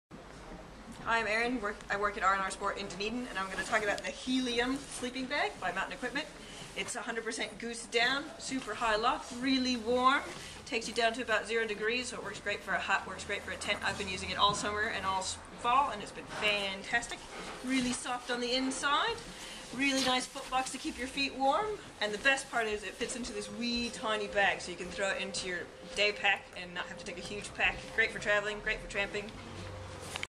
Mountain Equipment Helium 400 Sleeping Bag
If you're strictly a fair weather backpacker and mountaineer but want the reassurance of warmth on unseasonally chilly nights, then the Helium 400 could be the answer.